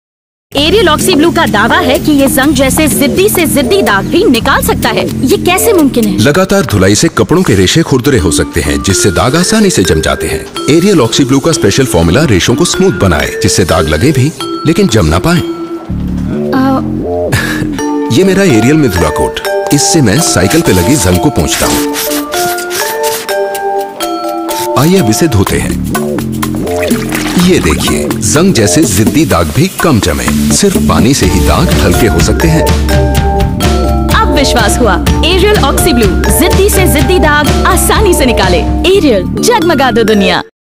ariel
This Beautiful and effective Ad film of ariel Directed by Nitish Sharma